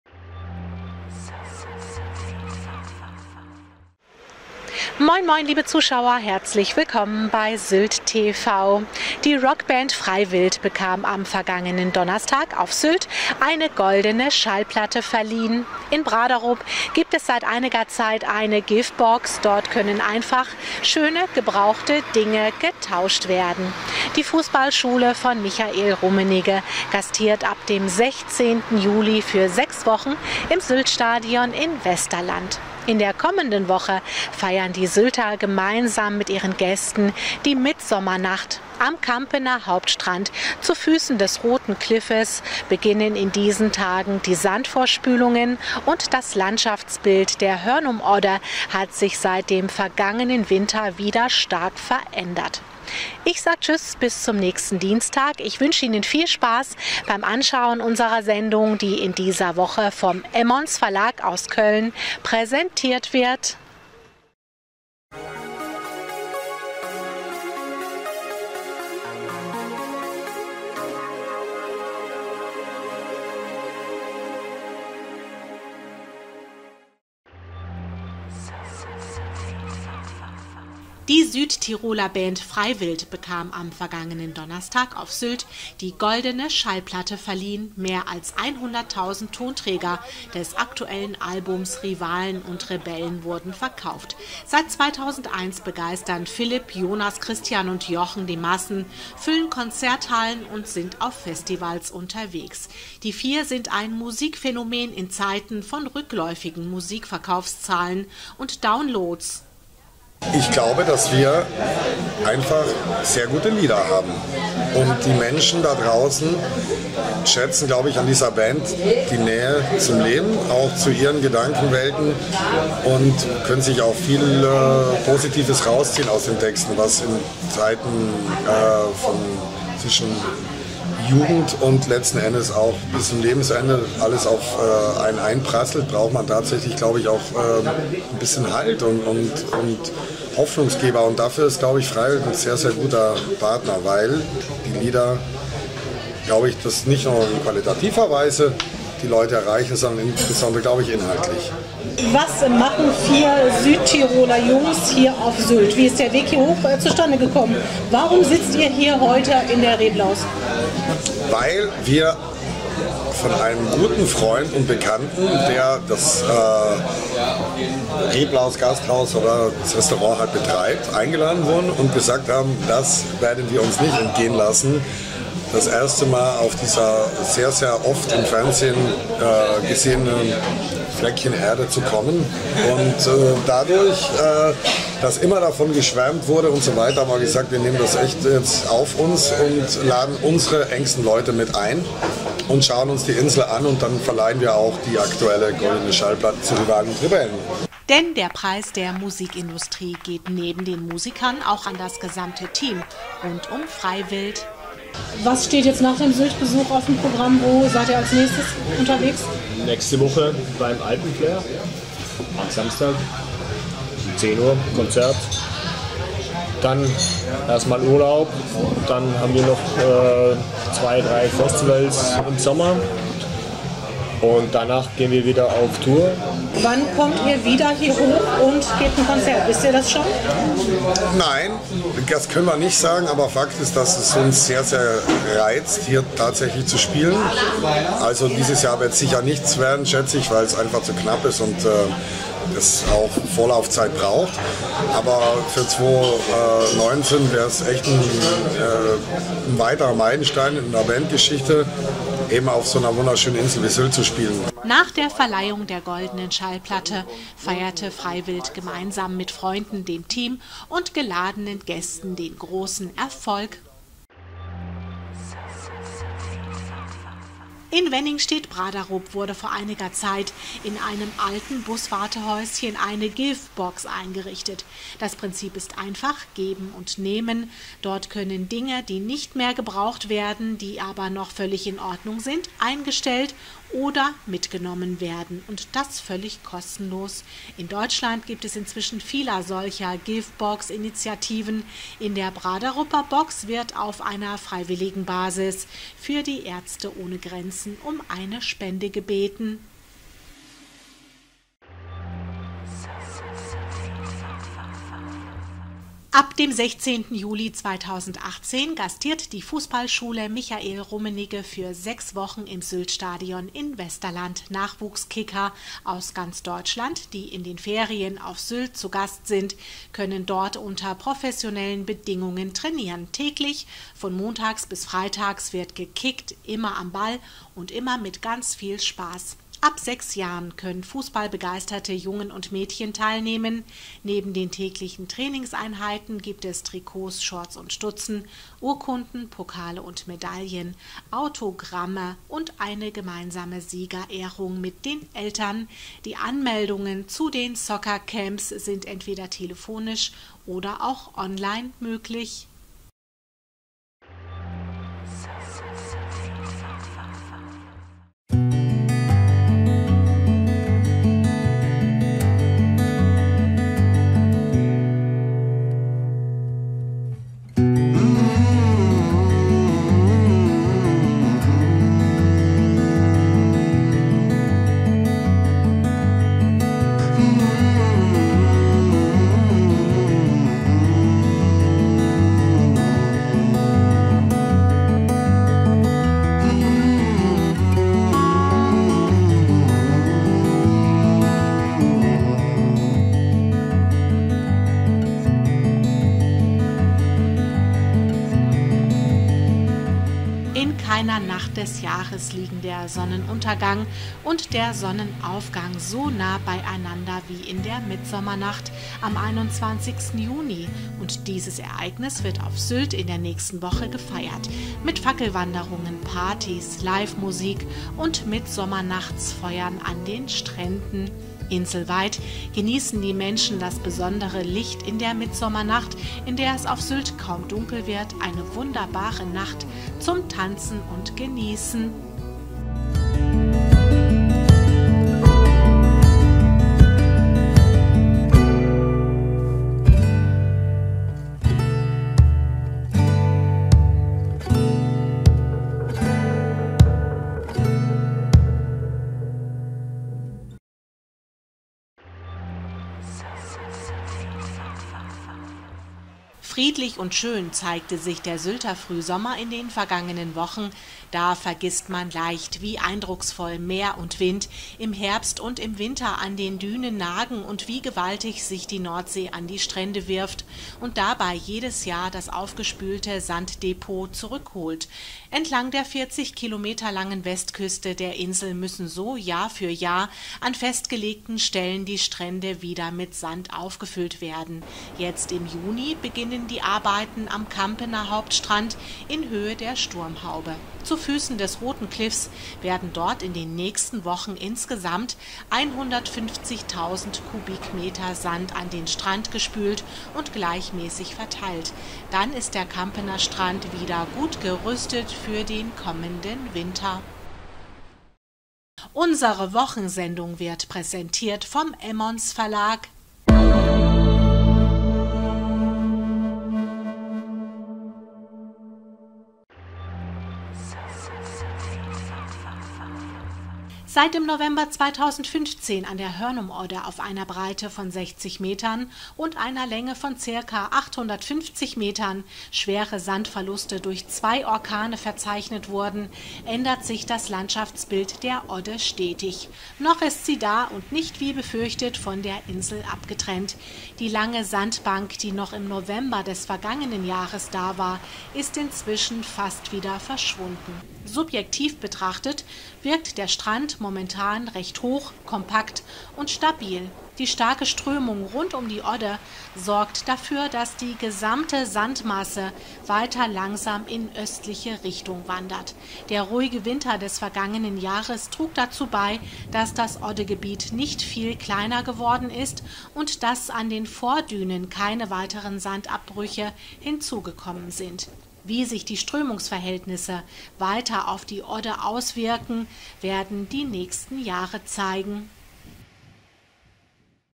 Sylt News 19. Juni 2018 - Die Sylt TV Woche
Die Sylt TV News der Woche mit folgenden Themen: Goldene Schallplatte für Rockband Frei.Wild, in Braderup wurde eine Give Box eingerichtet, die Rummenigge Fußballschule trainiert auch diesen Sommer wieder fußballbegeisterte Mädchen und Jungen, Sylt feiert die kürzeste Nacht des Jahres, in Kampen finden zurzeit Sandvorspülungen statt und der aktuelle Stand an der Hörnum Odde.